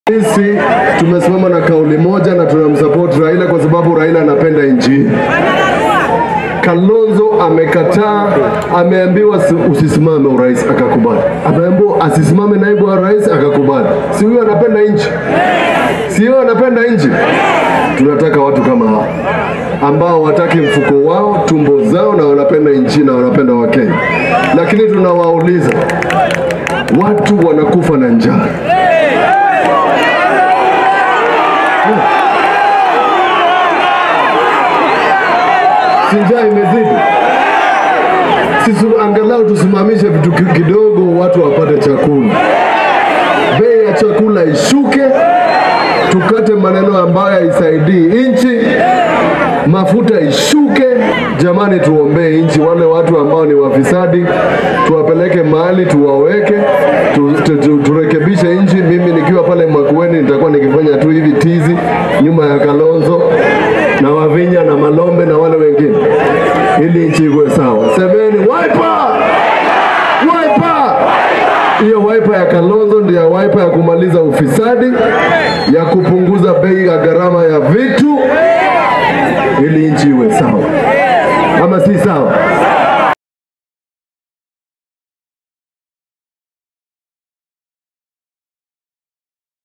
“Tukate maneno haisaidii nchi ndio bei ya chakula ishuke,” Mutula Kilonzo Jnr.
“Tukate maneno haisaidii nchi ndio bei ya chakula ishuke,” Mutula Kilonzo Jnr. drums up support for Raila